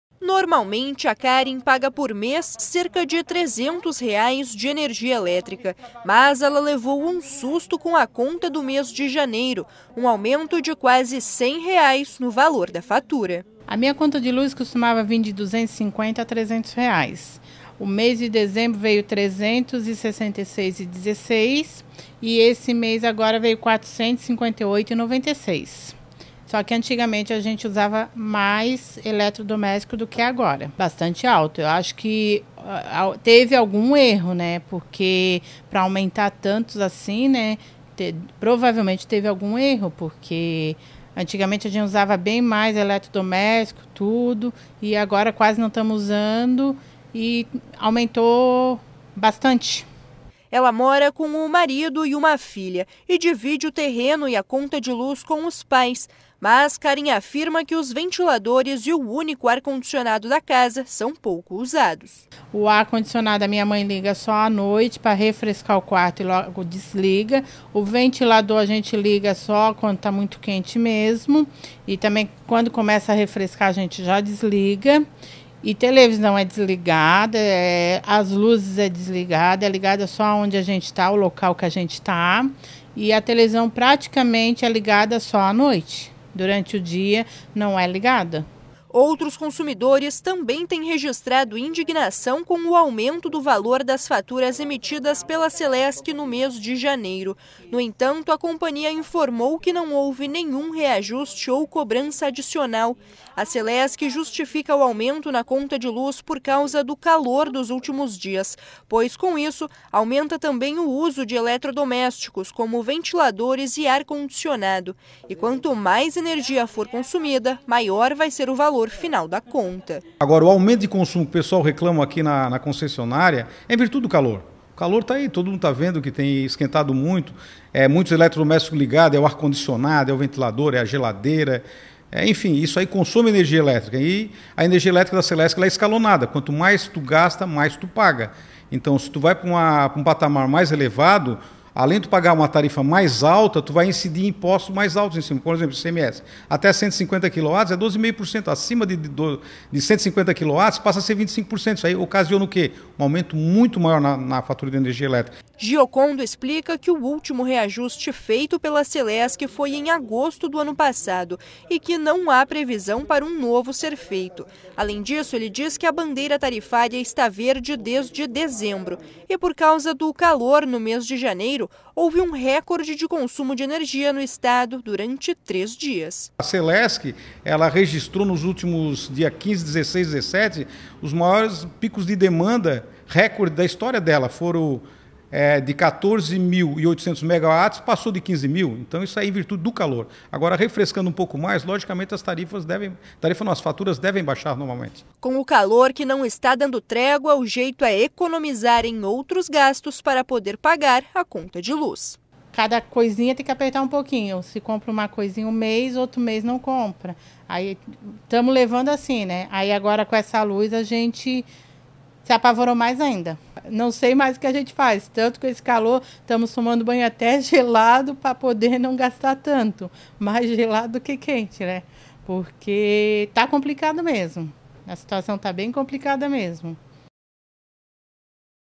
Consumidores reclamam de aumento no valor da conta de luz
Alguns consumidores têm percebido um aumento na fatura de energia elétrica emitida pela Celesc no mês de janeiro. No entanto a companhia diz que não houve nenhum erro ou reajuste de valores. E que a culpa é sim, do calor.